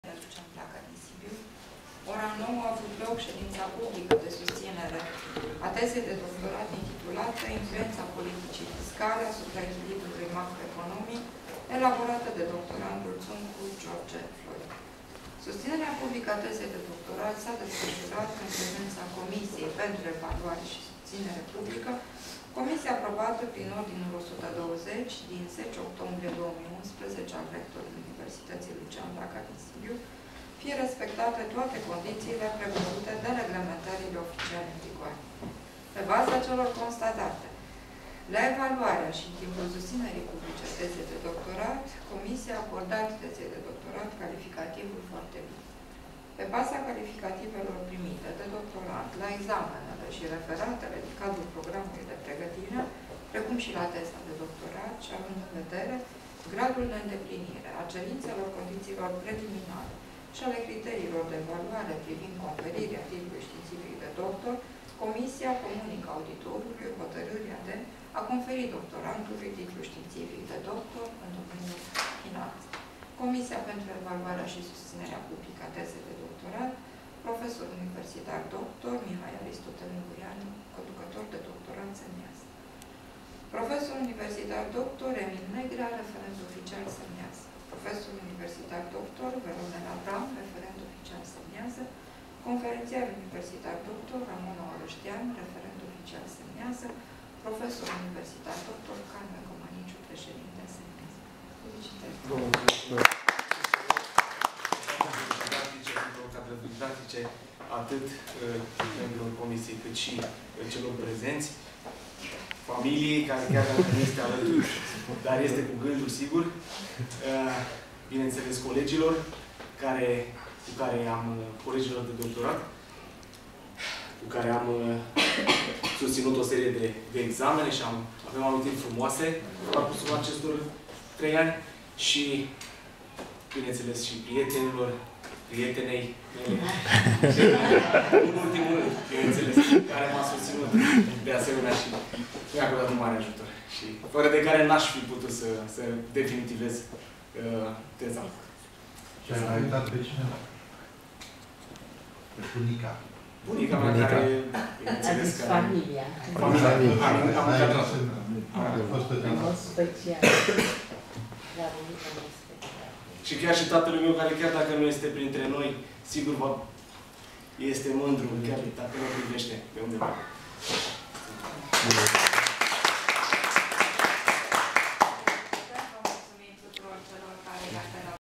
sustinere teza doctorat Asist.univ.dr.Ţuncu George Florin (incheiere)
Cuvintele de multumire...sper k nu am uitat pe cineva!! ;)) scuze anticipate!!
P.S. multumesc deasemenea tuturor colegilor de la Universitatea Constantin Brancoveanu!!! ...si in mod special domnului Rector (prof.univ.dr. Puiu Alexandru) si doamnei Decan ( prof.univ.dr. Ciochina Iuliana).